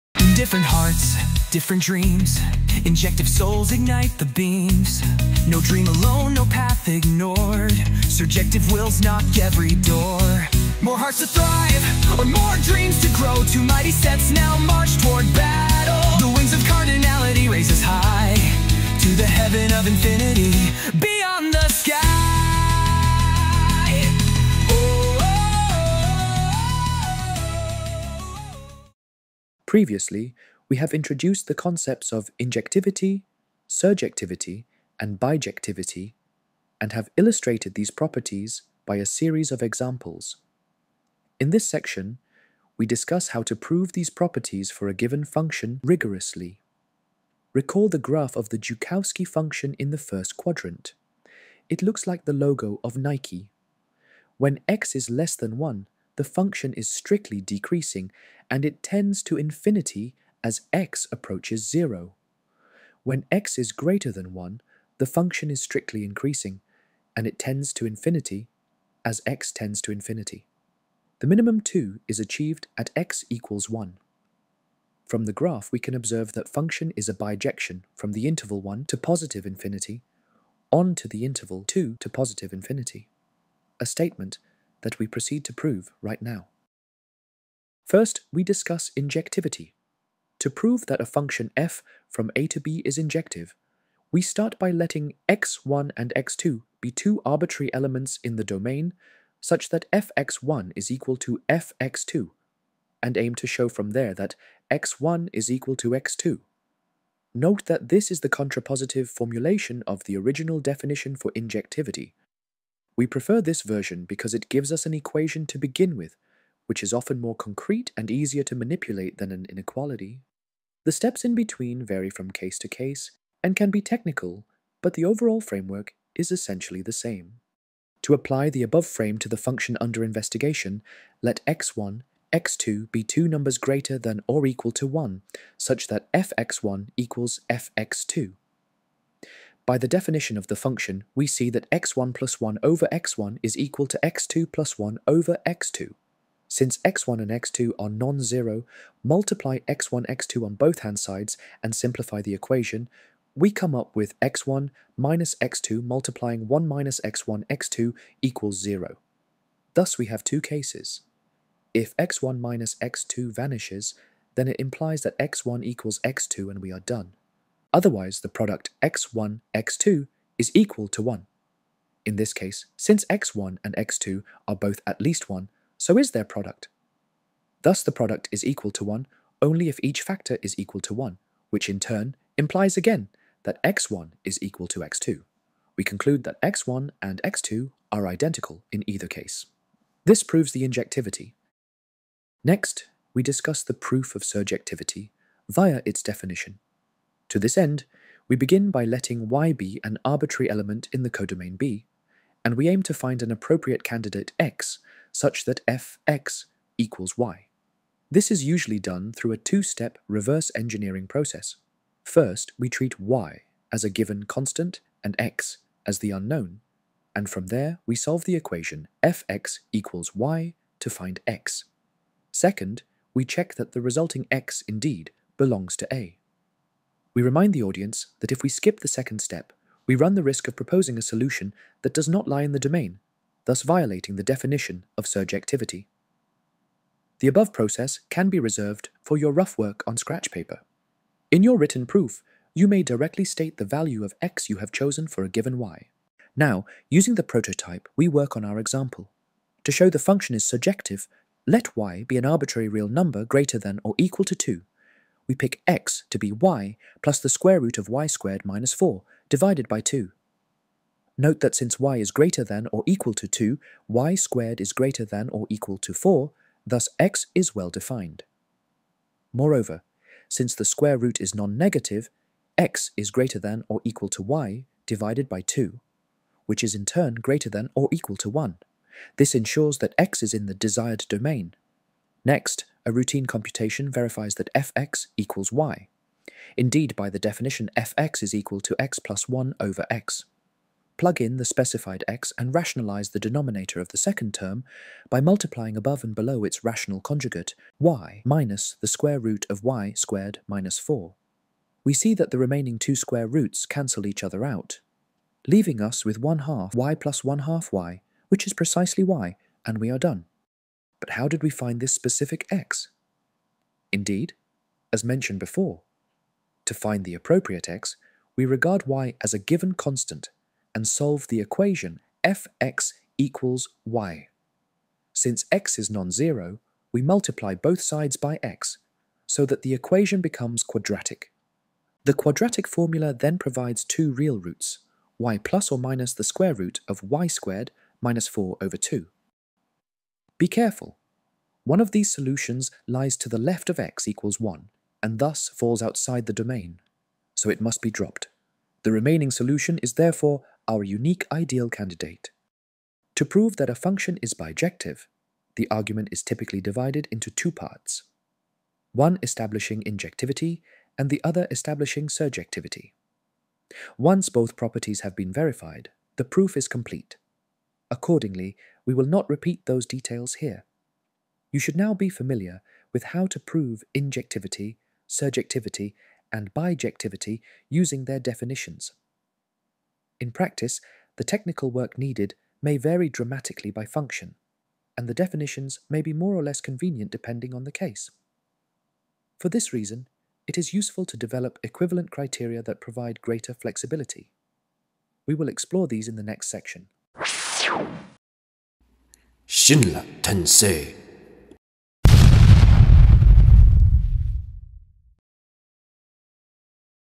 Injectivity, Surjectivity and Bijectivity -- 2. Proof Format
This video introduces how to write proofs for injectivity, surjectivity, and bijectivity of a given function, based on the their definitions.